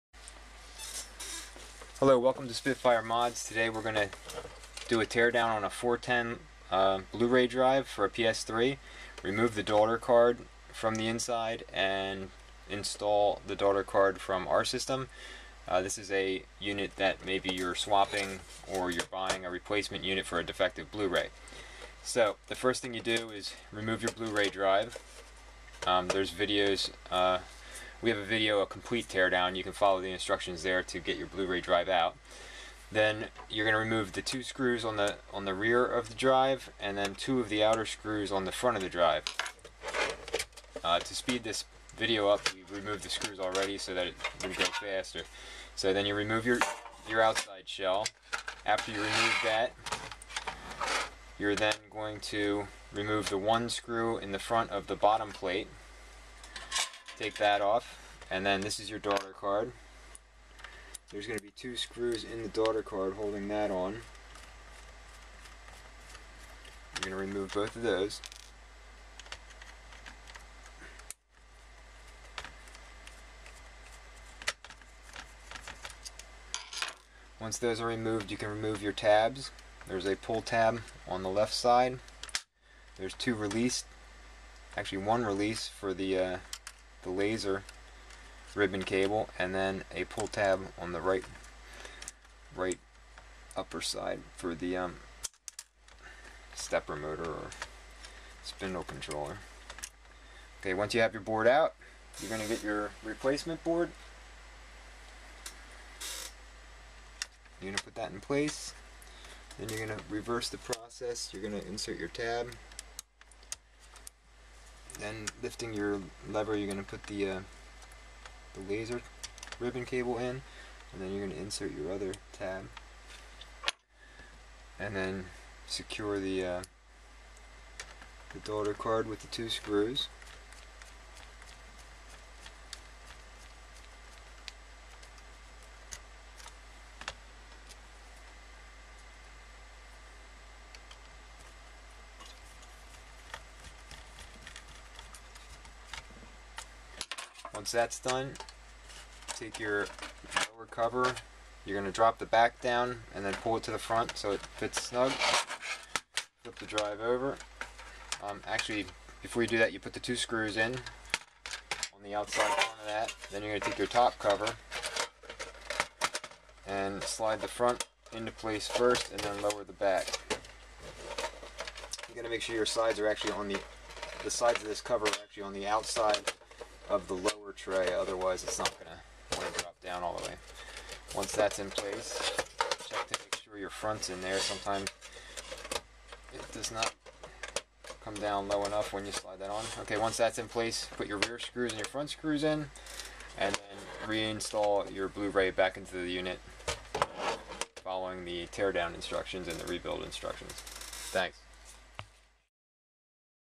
PS3 Blu Ray Fix 410 Model Swap out Drive by changing the daughter card.
Example of Blu Ray repair for bad laser on a PS3 or Playstation 3.